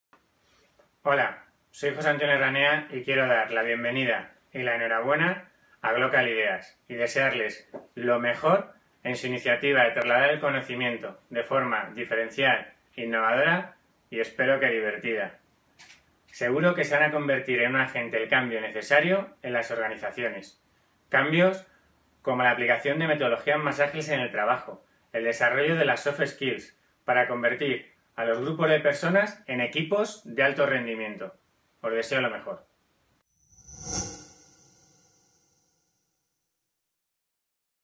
Saludo José Antonio Ranea/ Gestión logística de gas
José Antonio Ranea, responsable de gestión logística de gas en Endesa, envía un saludo a Glocalideas.